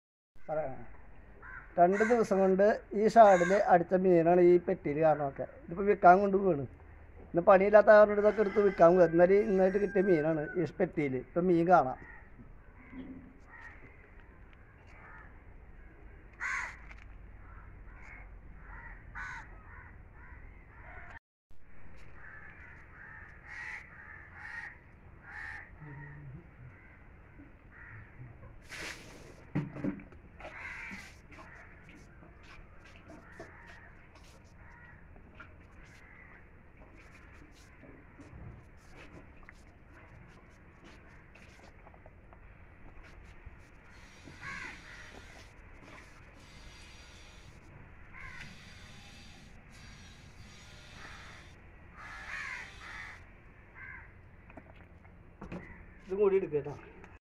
ചൂണ്ടയിൽ പിടിച്ച മീനുകളെയെല്ലാം കൊടുത്തു ഗയ്സ്
നമുക്ക് കിട്ടിയ മീനുകളെയെല്ലാം കൊടുത്തു ഗയ്സ്